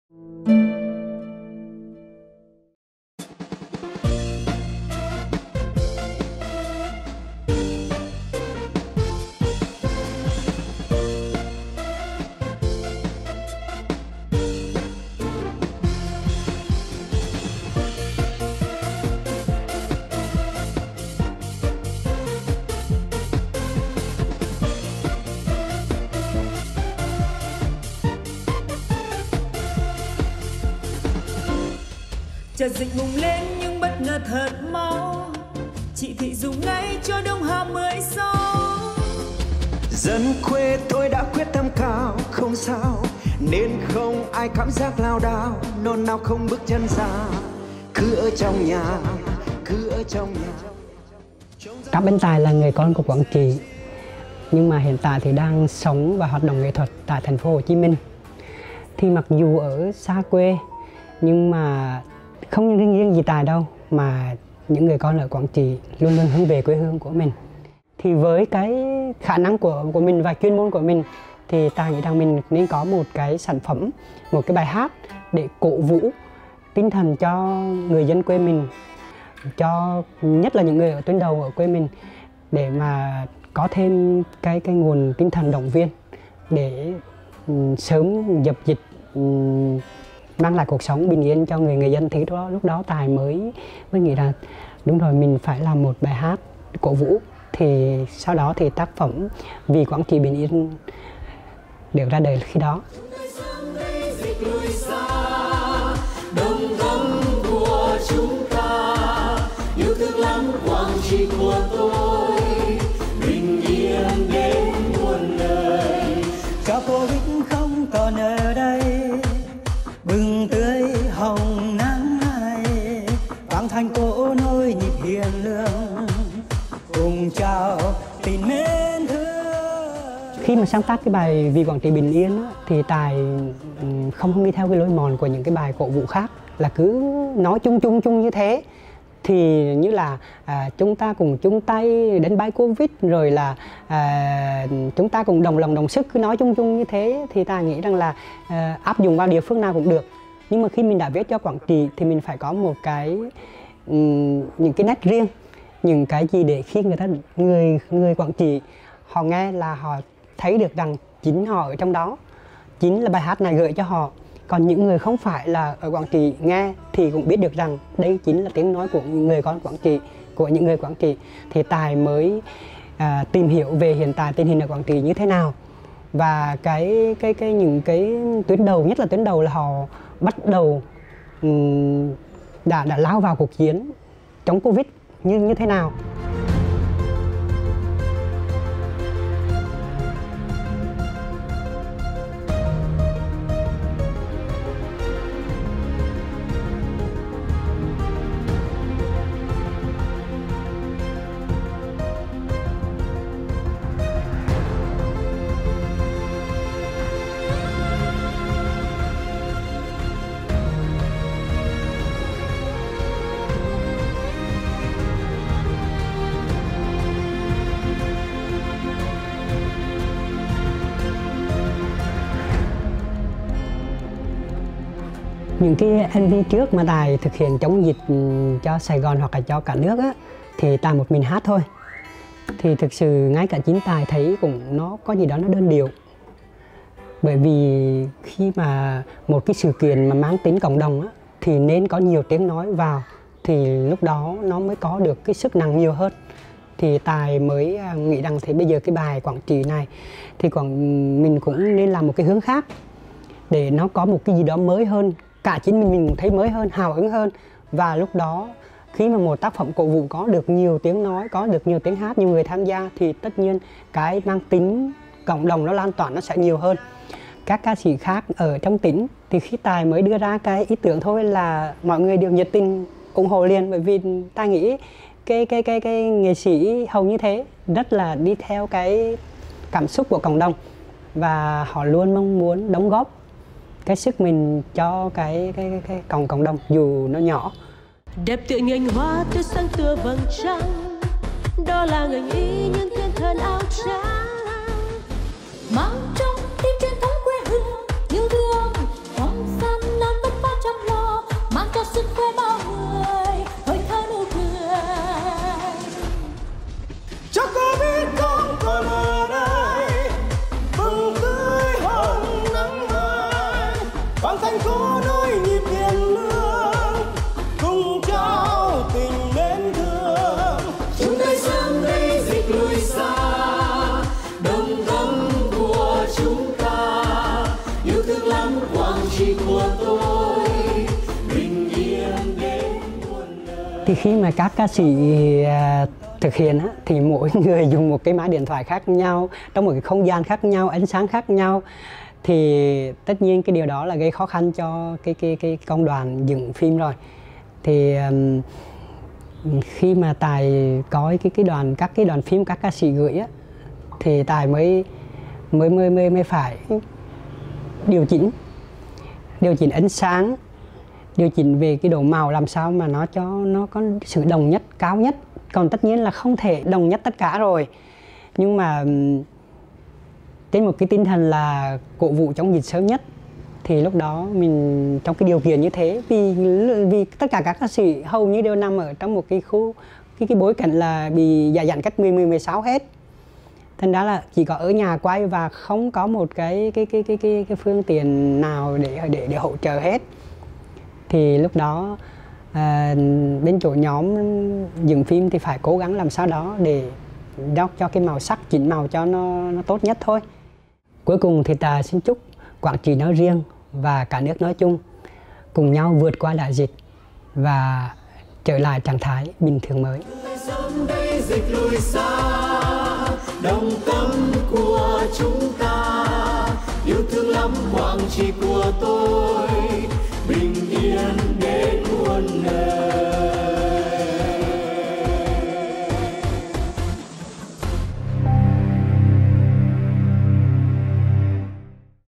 Nhạc sĩ Quảng Trị sáng tác ca khúc cổ vũ quê hương chống dịch
Khi làn sóng dịch Covid-19 thứ tư ập đến, nhìn quê hương phải chống chọi với dịch bệnh, nhạc sĩ - ca sĩ Cáp Anh Tài - một người con của Quảng Trị, đã đau đáu phải viết một ca khúc để cổ vũ tinh thần cho người dân quê mình, đặc biệt là cho tuyến đầu chống dịch.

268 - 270 Nguyễn Đình Chiểu, Phường Võ Thị Sáu, Q.3, TP HCM